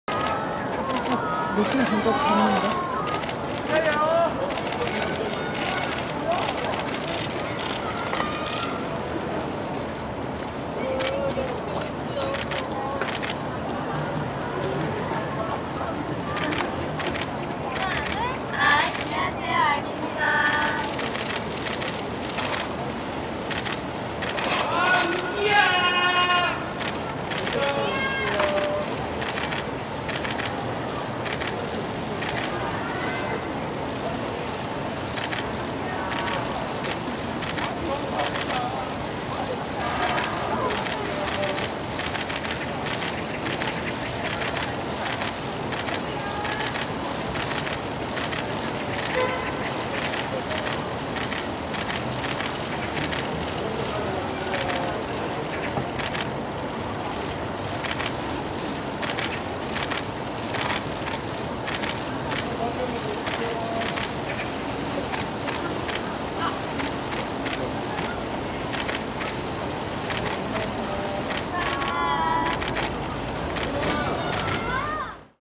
(여자)아이들 @ KBS MUSIC BANK 출근길
3월 8일 오전 KBS 뮤직뱅크에 (여자)아이들이 리허설 녹화에 참여하기 위해 출근하고 있다. 뮤직뱅크 3월 8일 출연자는 (여자)아이들, ITZY, Jus2, MONSTA X, N.Flying, SF9, TOMORROW X TOGETHER, VANNER (배너), 드림캐쳐, 라비 (RAVI), 온앤오프 (ONF), 용감한 홍차, 임팩트, 자이언트핑크, 장동우, 트레이, 하성운, 하은요셉이다.